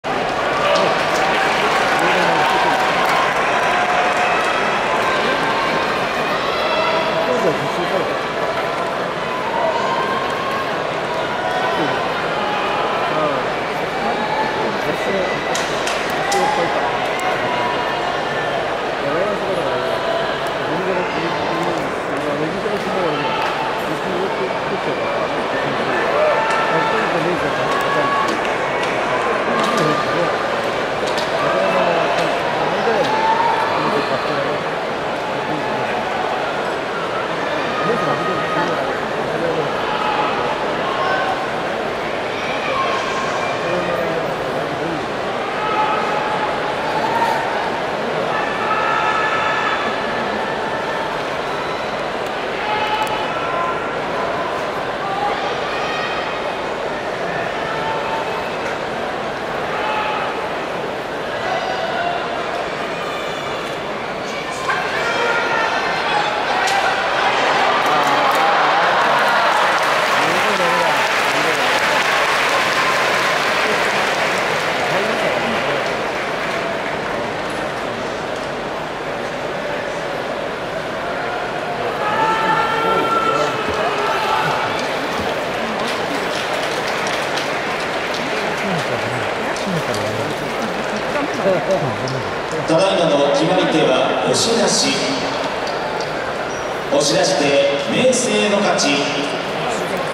琴恵光 vs 明生【大相撲令和5年9月場所】千秋楽 2023/9/24 Kotoeko vs Meisei [Grand Sumo] DAY15 SEP basho 2023 現地観戦 秋場所
Channel registration↑↑↑